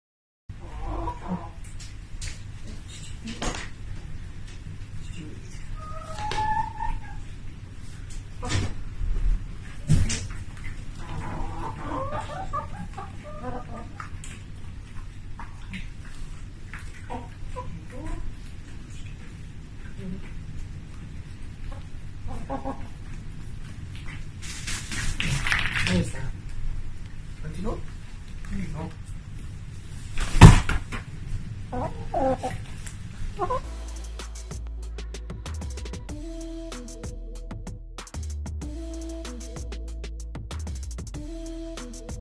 how to create a bathing tab for your caged chickens
Chickens need to bathe in soil to clean themselves but most importantly to remove pests that hide in their feathers.